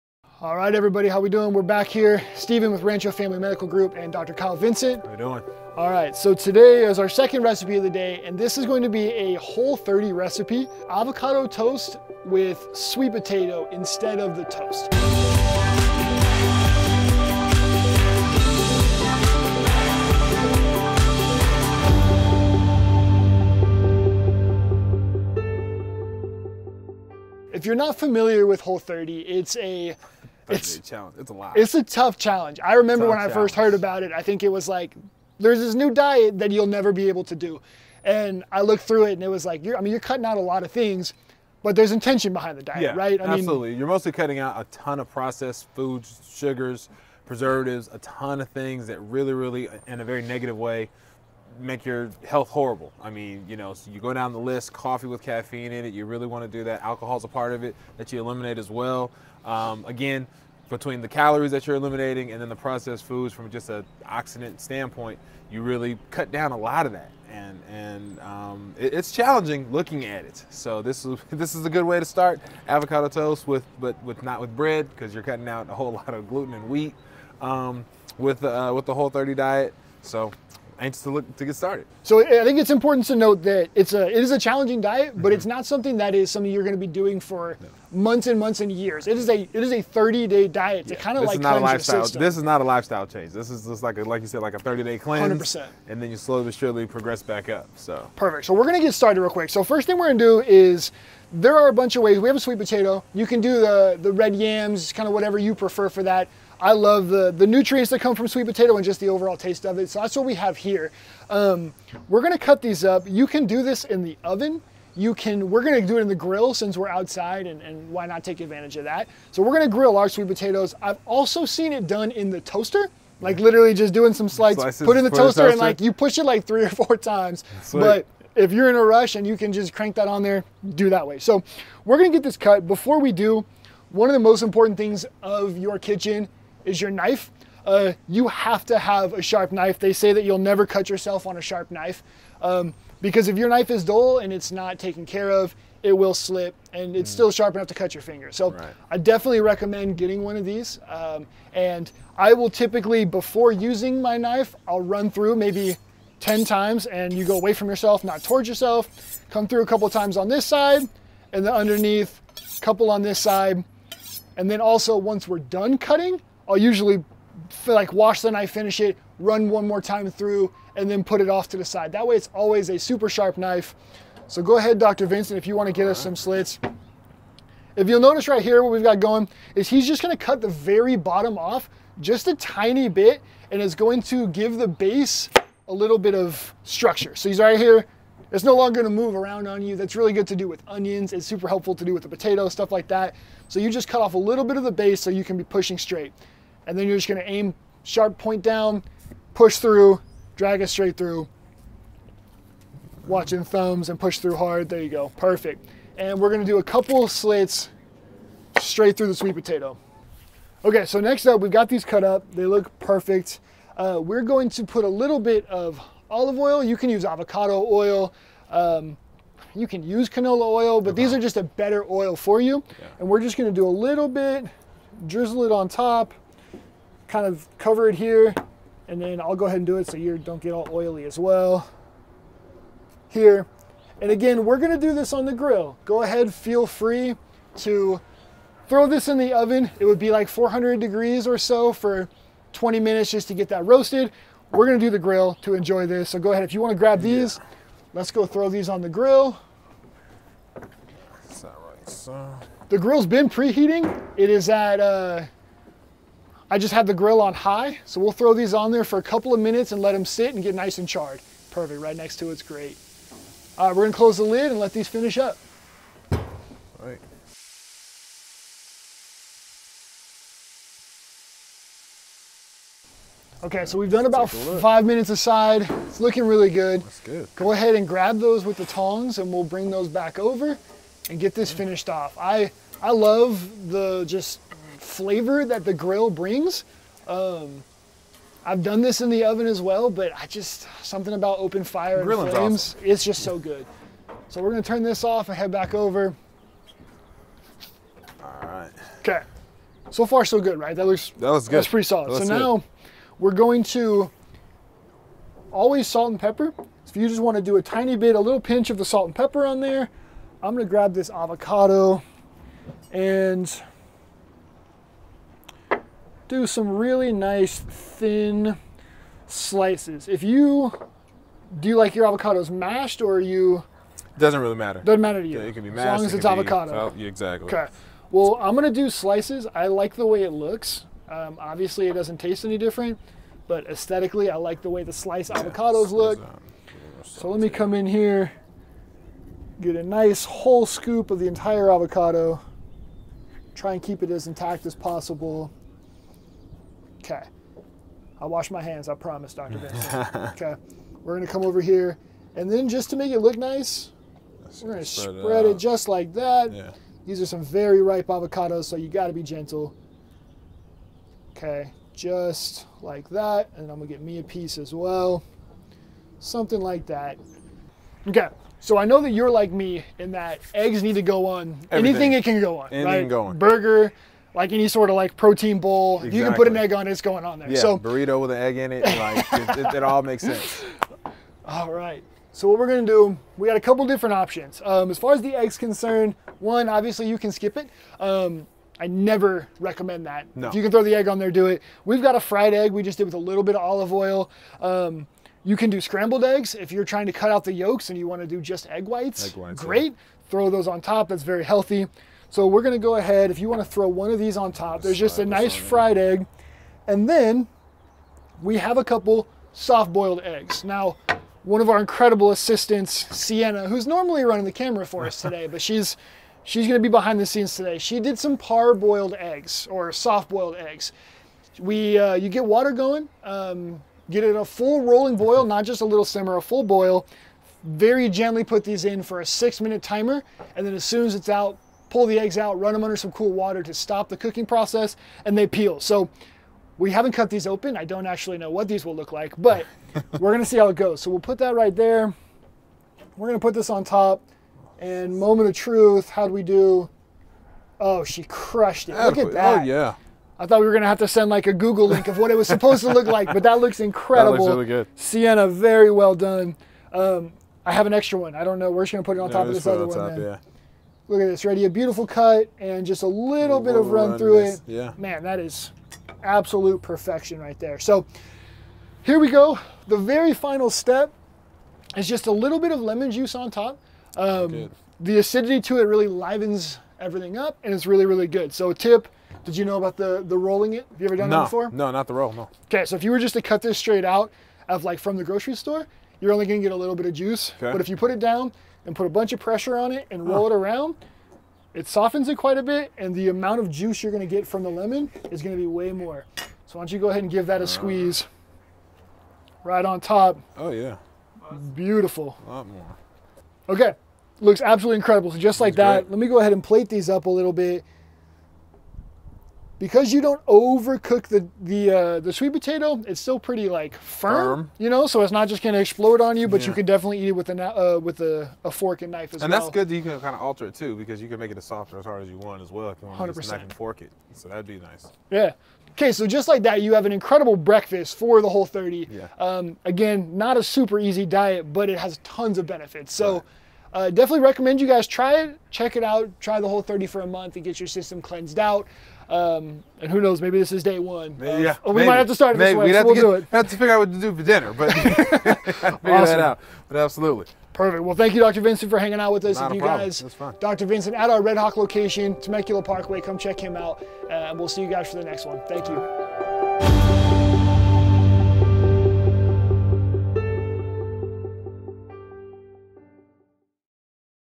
Sweet Potato Avocado Toast with Dr. Kyle Vincent
In this video, Dr. Kyle Vincent shows us how to make a Sweet Potato Avocado Toast! Check out the link below to find out more about Dr. Vincent: